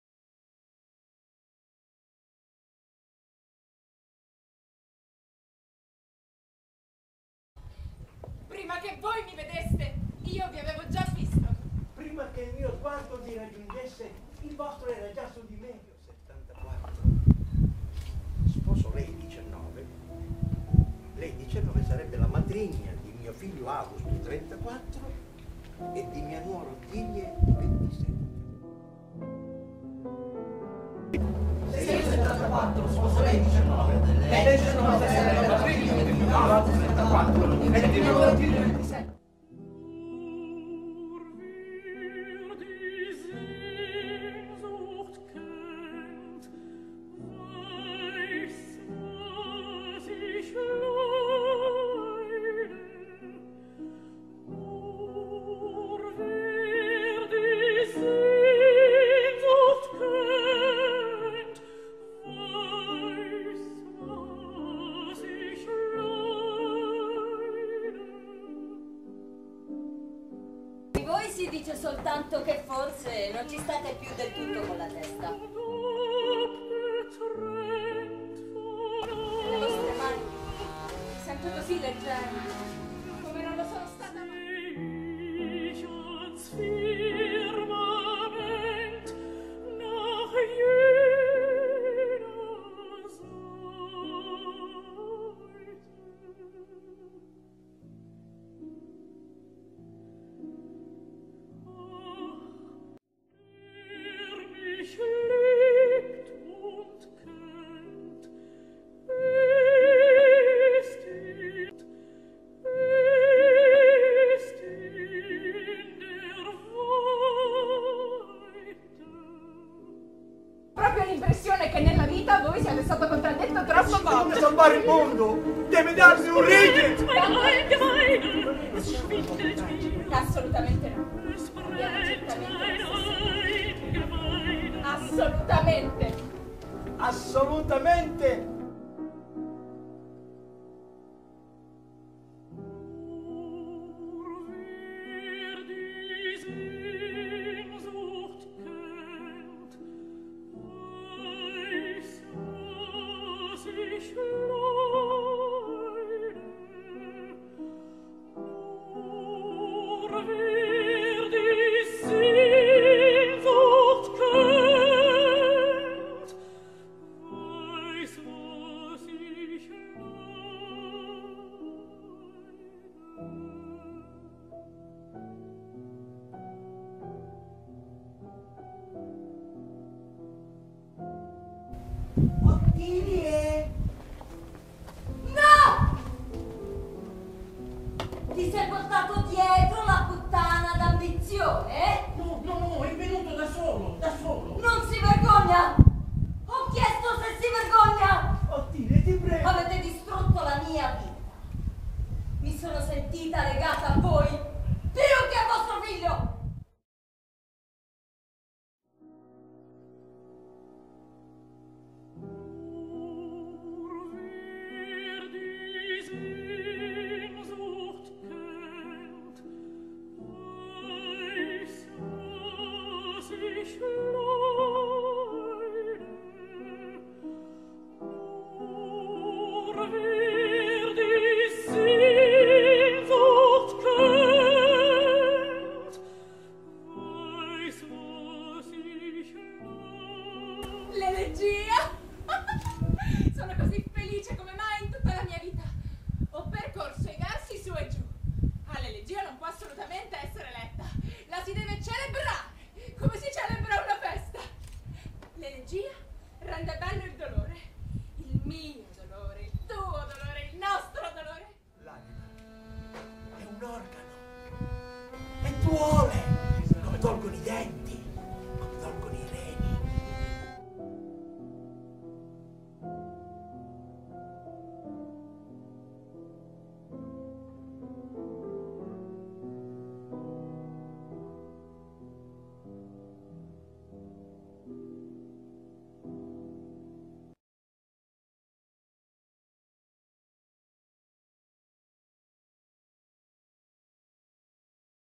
Martin Walser "Un uomo che ama" (Ein liebender Mann) - Trailer
Messa in scena a Catania , presso Castello Ursino del dramma di Martin Walser - il 12 13 e 14 luglio 2013 con la regia di Elio Gimbo.